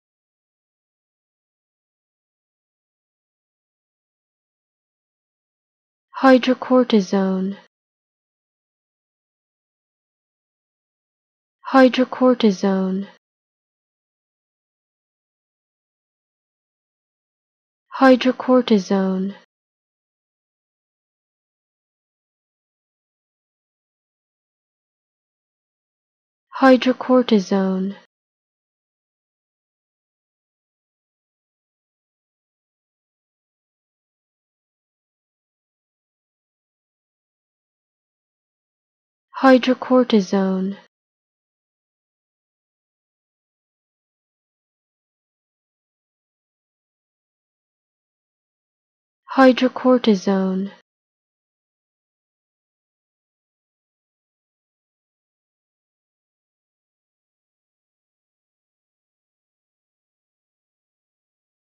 How To Say Hydrocortisone
Learn how to say Hydrocortisone with EmmaSaying free pronunciation tutorials.